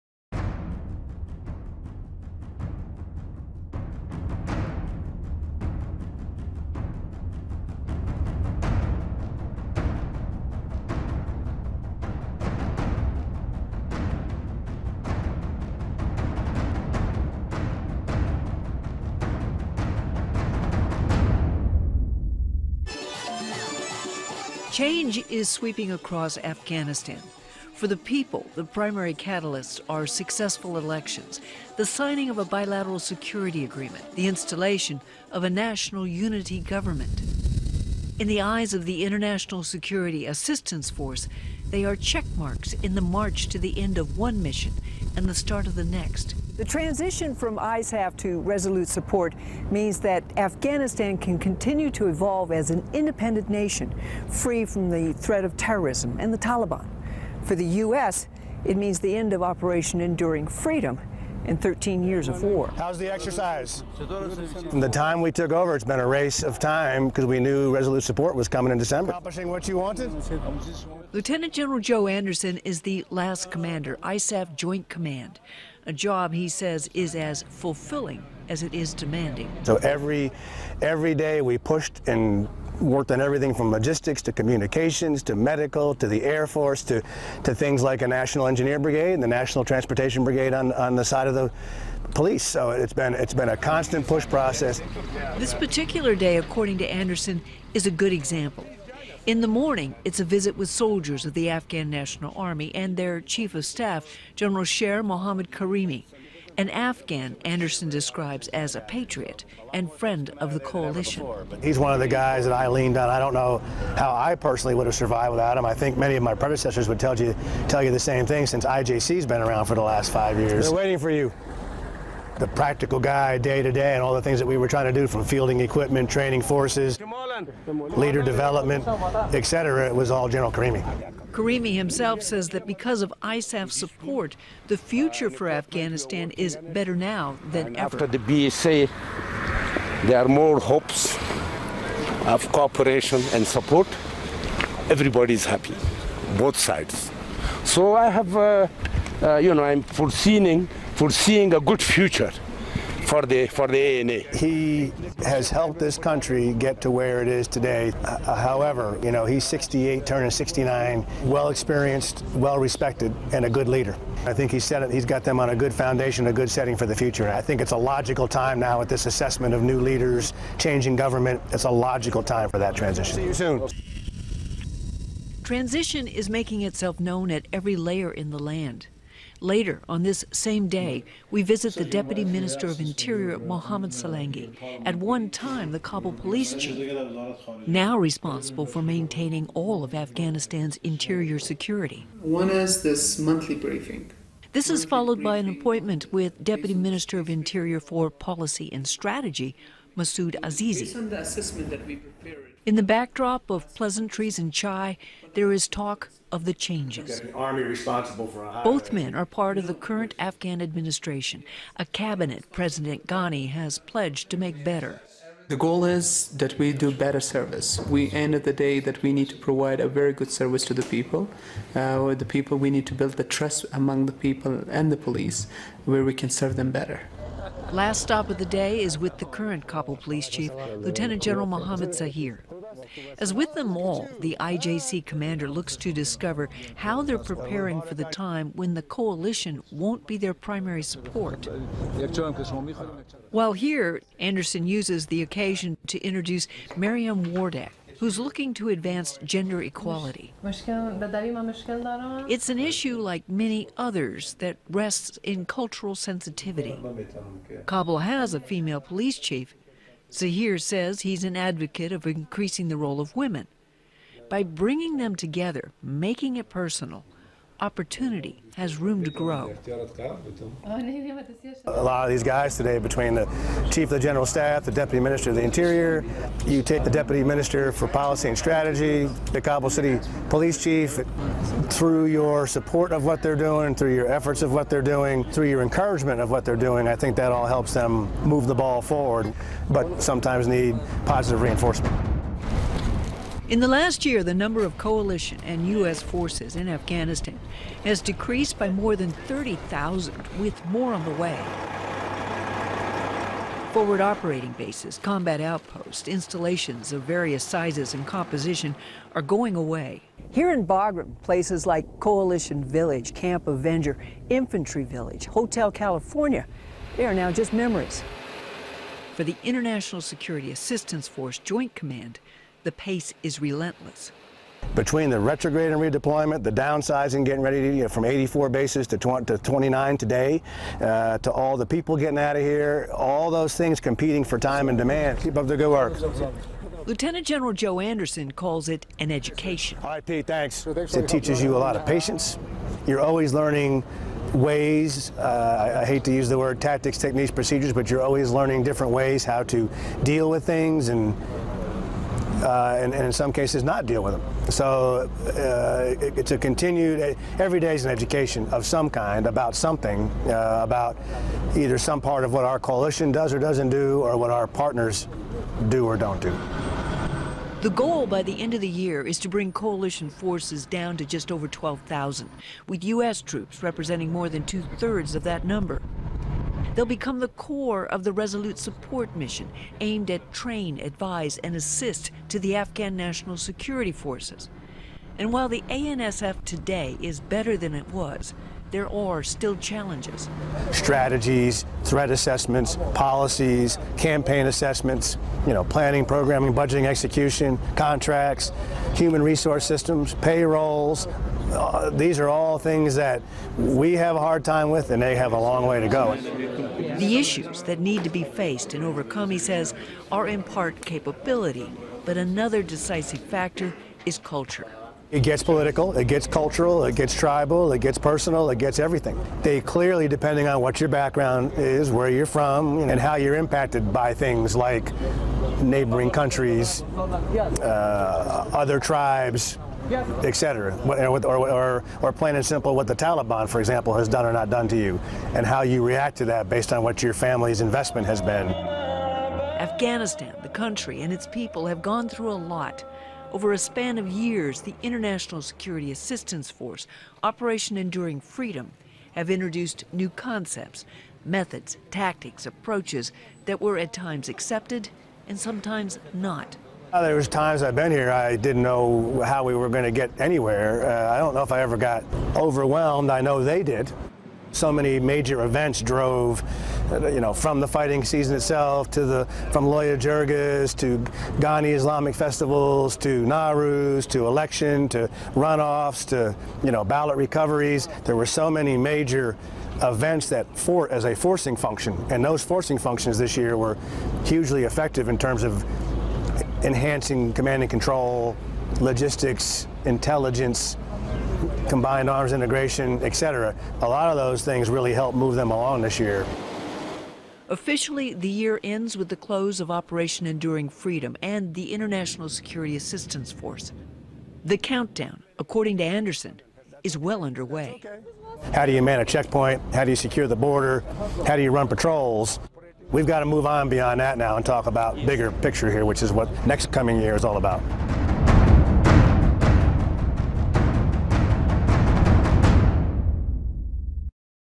A Closer Look: A Day in the Countdown
The push is on: re-balance the footprint and make sure Afghan National Security Forces are ready. Join Gail McCabe as she travels with Lt. Gen. Joe Anderson, Commander, ISAF Joint Command, in "A Closer Look: A Day in the Countdown."